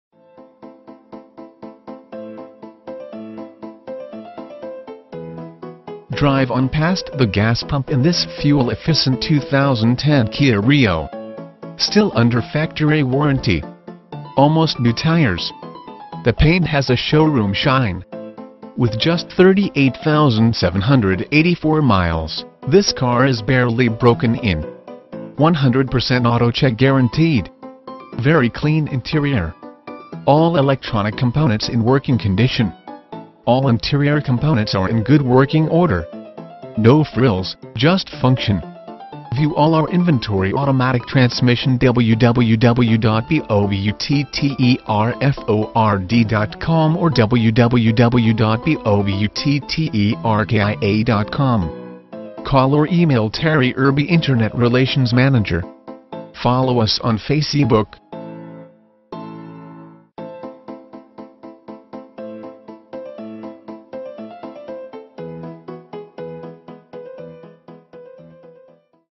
2010 Kia Rio - Blake Utter Ford
215 N. Hwy 75

For Sale: 2010 Kia Rio
VIN: KNADH4A31A6615100
Engine: 1.6L DOHC MPFI CVVT 16-valve I4 engine
Drivetrain:
Transmission: Automatic
Mileage: 38,784
Color: Gold (exterior) Beige (interior)

Features:
Drive on past the gas pump in this fuel efficent 2010 Kia Rio!  Still under factory warranty!  Almost new tires. The paint has a showroom shine. With just 38784 miles, this car is barely broken in. 100% AutoCheck guaranteed! Very clean interior! All electronic components in working condition. All interior components are in good working order. No frills, just function.  View all our inventory automatic transmission